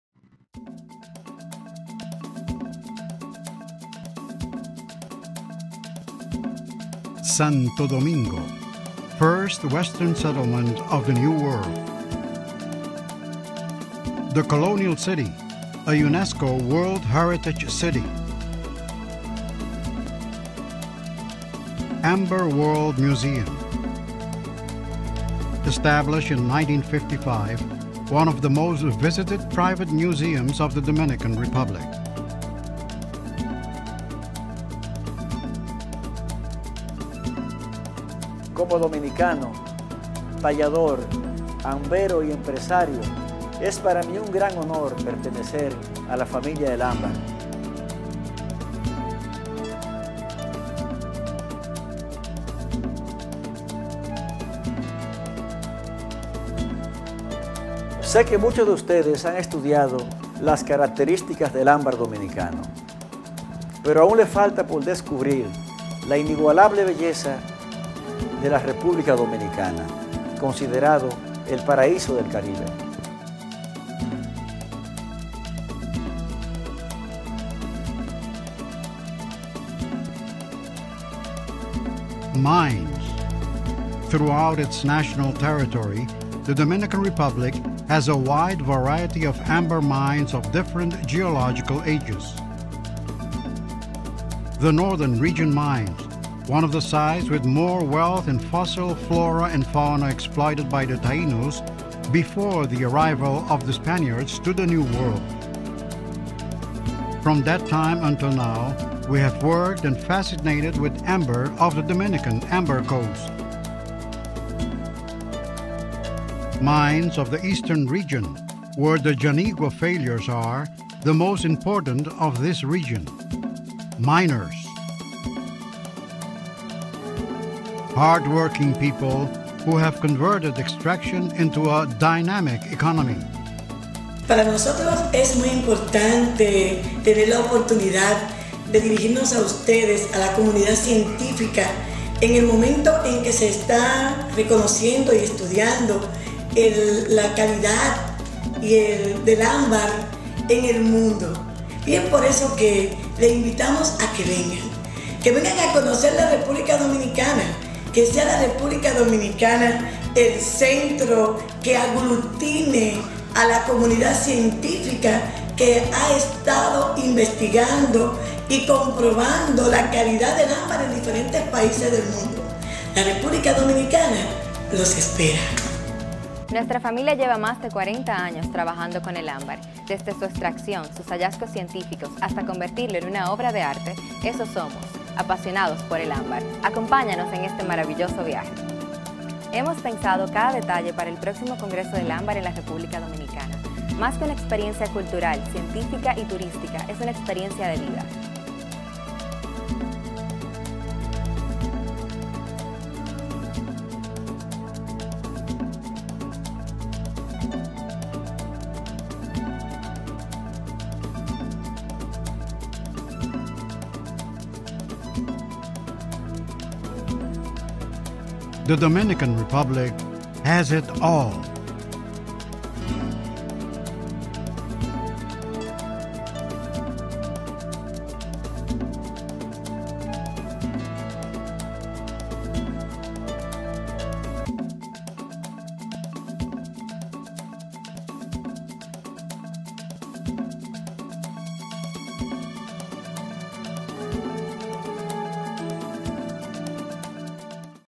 Next Amber Conference in Santo Domingo April 8th -12th, 2019
Amber World Museum invites you to participate in the 8th International Conference on Fossil Insects, Arthropods and Amber in the City of Santo Domingo.